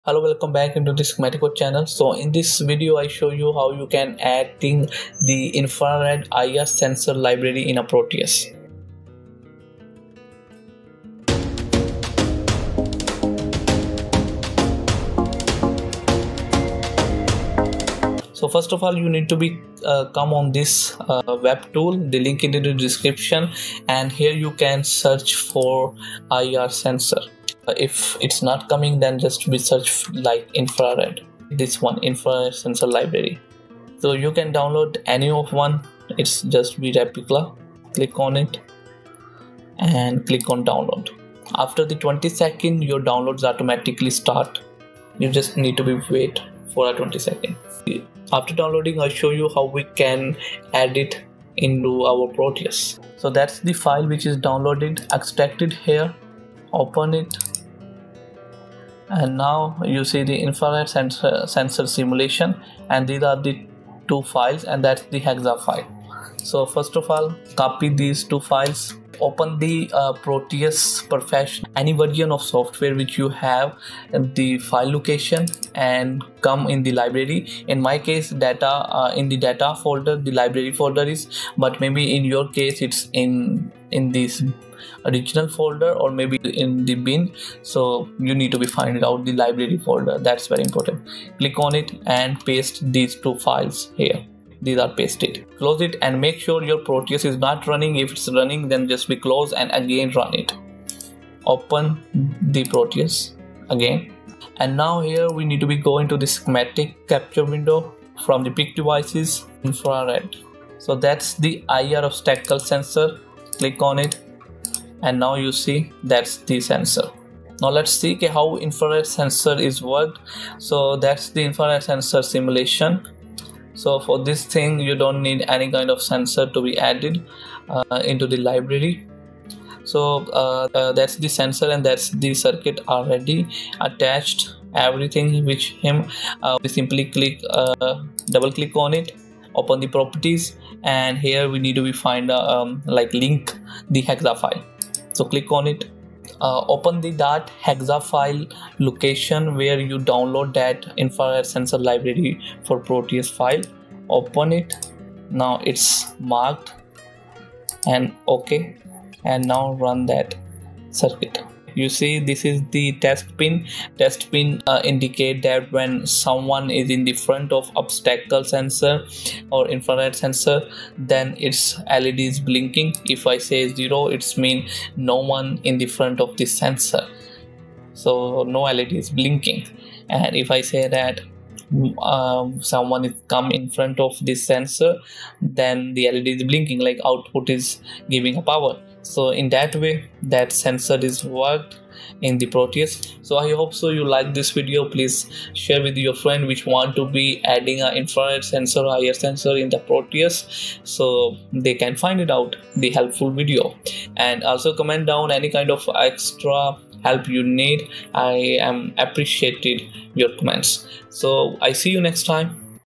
Adding Infrared (IR) Sensor Library to Proteus 8.0 to 9.0 in 2025 [ Download, Add & Explain ]
This video tutorial will show you how to add the Infrared (IR) Sensor Library to Proteus 8.0 to 9.0 in 2025 — including the complete process to download, install, and use it for your circuit simulations. Infrared (IR) sensors are essential components in projects like object detection, motion sensing, remote control systems, and automation.

In this step-by-step guide, you’ll learn how to:
Download the latest IR Sensor Library (2025 version)
Add it properly to Proteus 8.0, 8.9, and 9.0
Simulate IR-based circuits and understand sensor operation in Proteus

Whether you’re a beginner or advanced electronics student, this video will help you easily integrate and test Infrared sensors in your Proteus simulations.